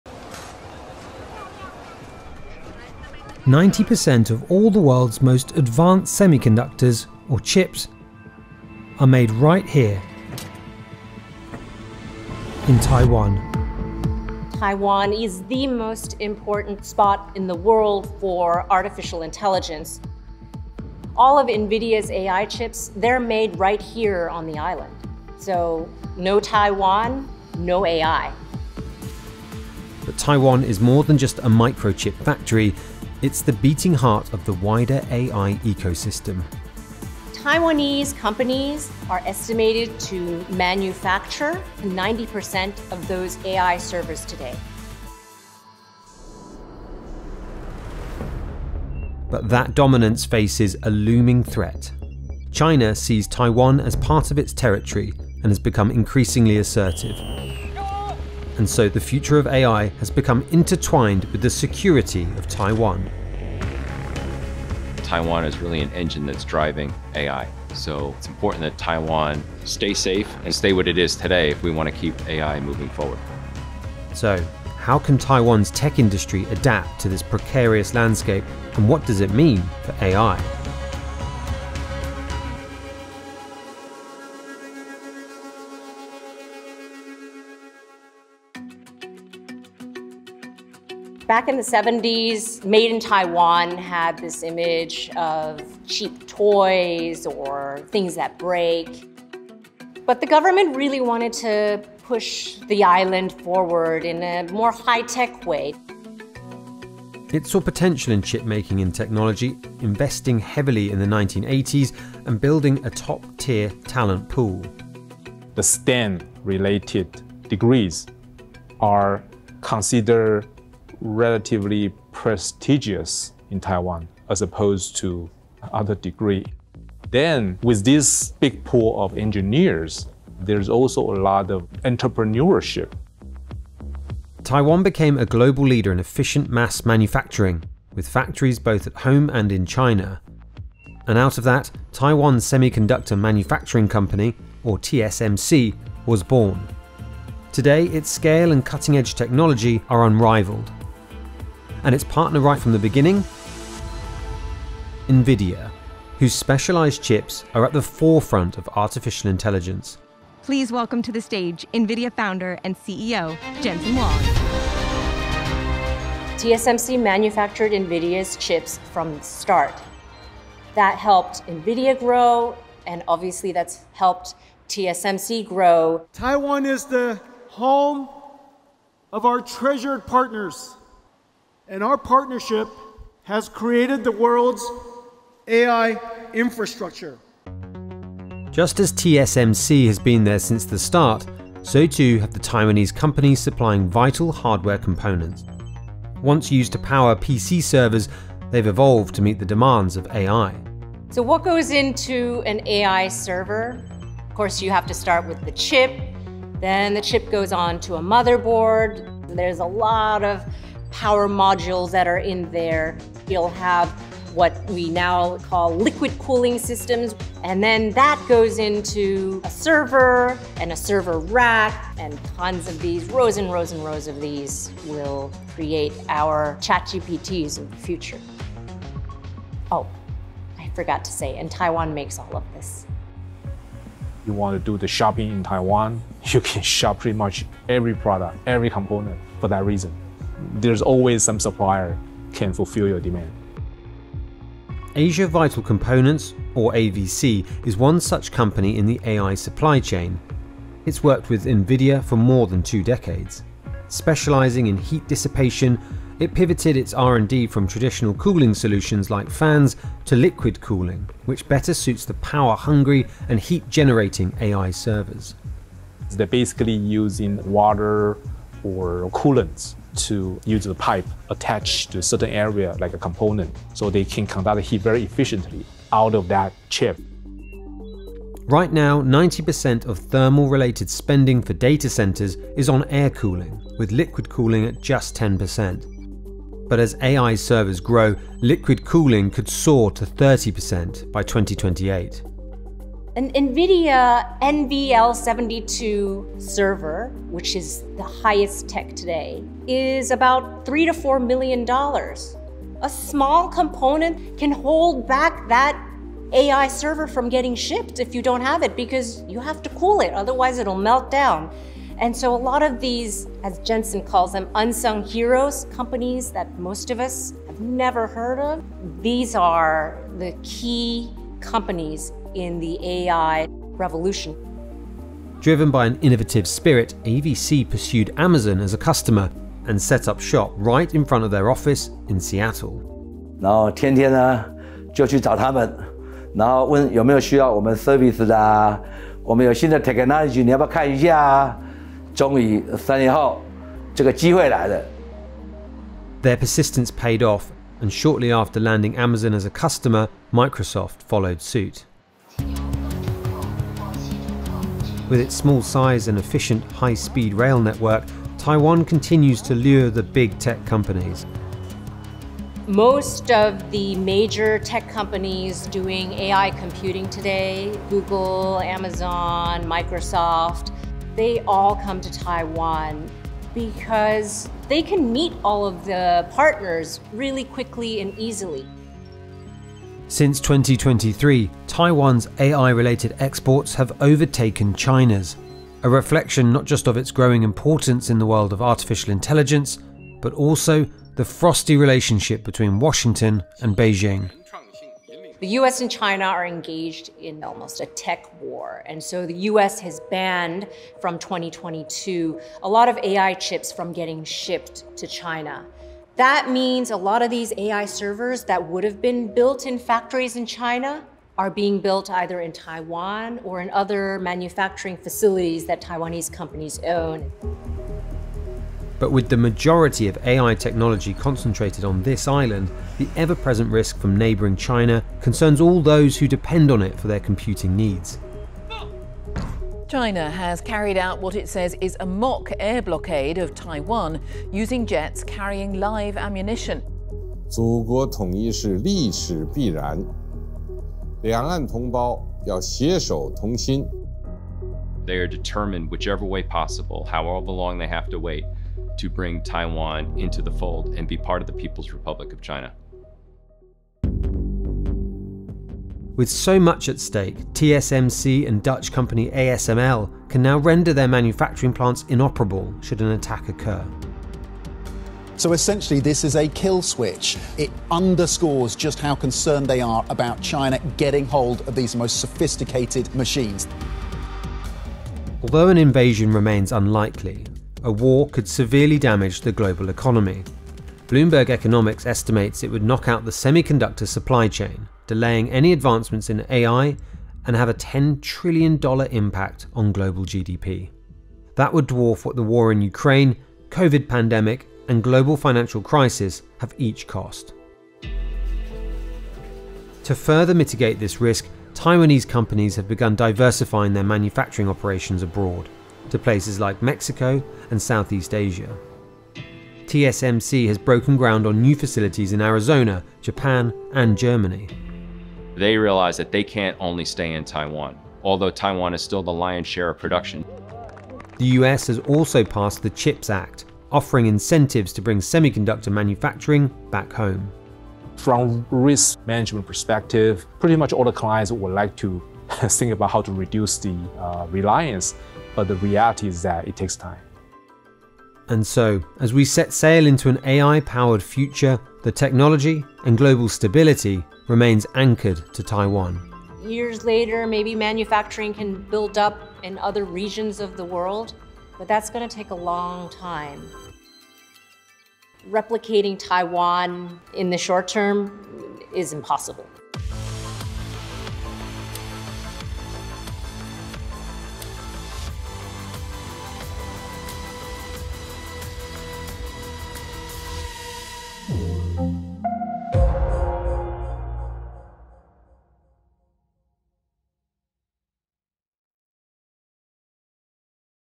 Why AI Can't Exist Without Taiwan
Right now, the world depends on Taiwan for almost all of the critical hardware needed to power AI. But how is its tech industry adapting to the growing threat of conflict with Beijing?

00:00 Intro
01:28 History of Taiwan’s Tech Dominance
03:07 Taiwan’s Unsung Heroes - AI Hardware Gems
04:04 Shifting Technology Demands for AI
05:52 Why Taiwan?
06:57 Geopolitical Risks to the Supply Chain
08:20 What’s next?

Get unlimited access to Bloomberg.com for $1.99/month for the first 3 months